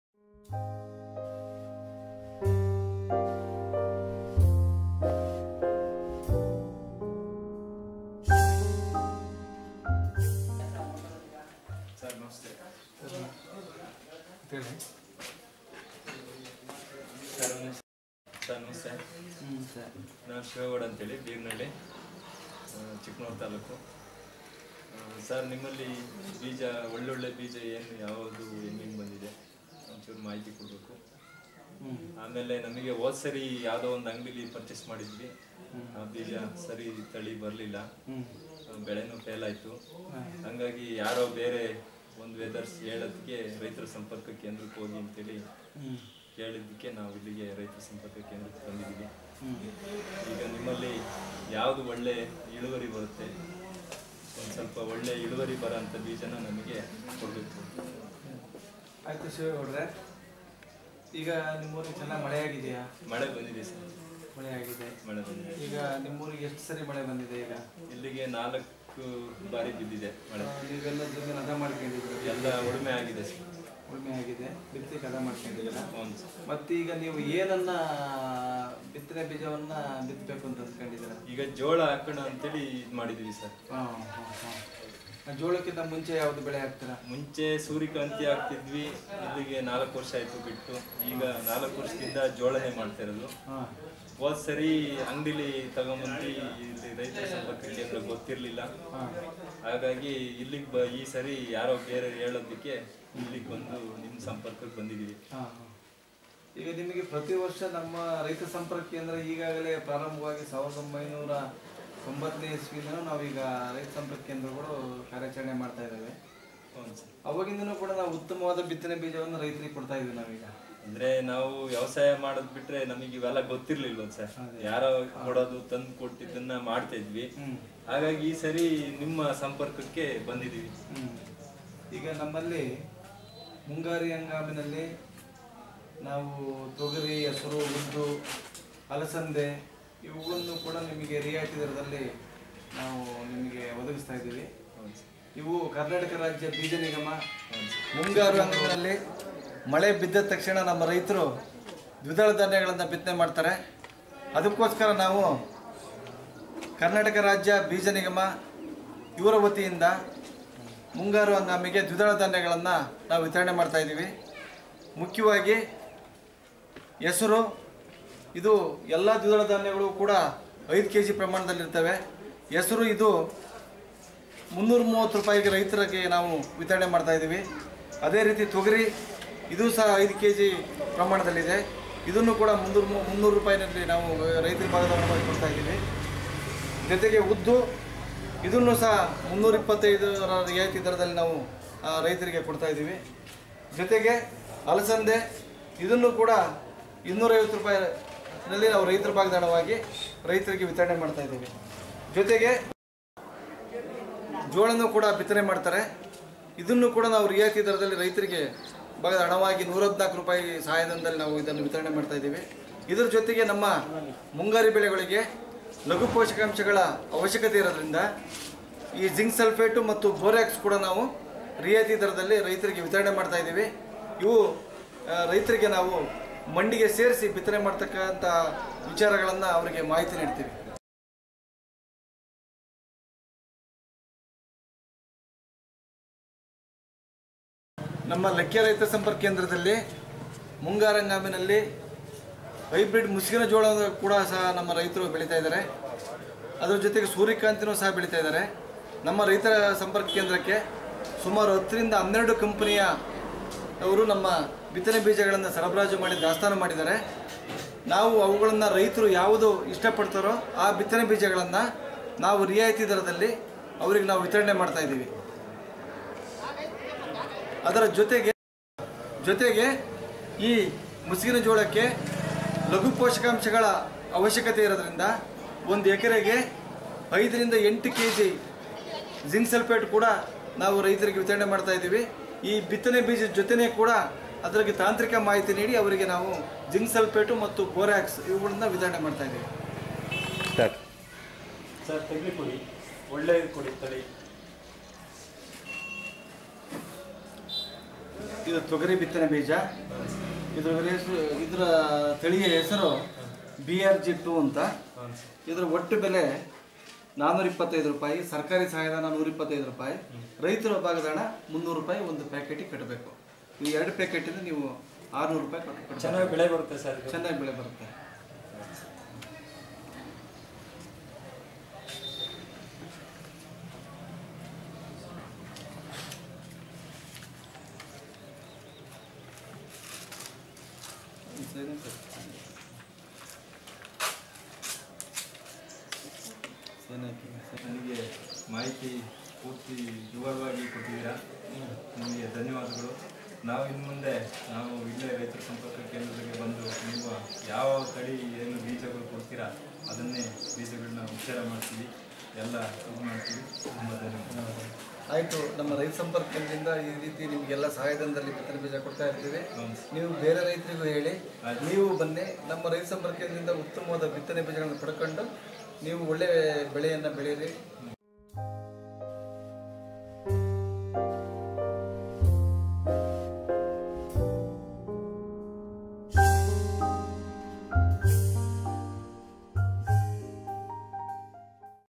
Seeds availability in Raitha Samparka Kendra
This video was produced in Chikkamagalur and the video explains the availability of different seeds in Raitha Samparka Kendra. For more information and related videos visit us